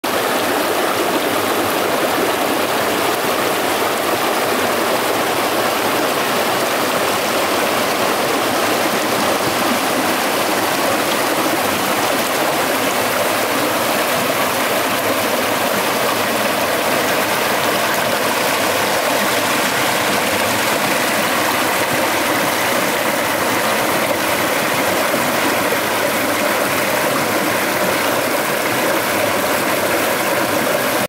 The sights and sounds of a creek in the spring. ☮️💕
Just a nice spot to kick back and relax.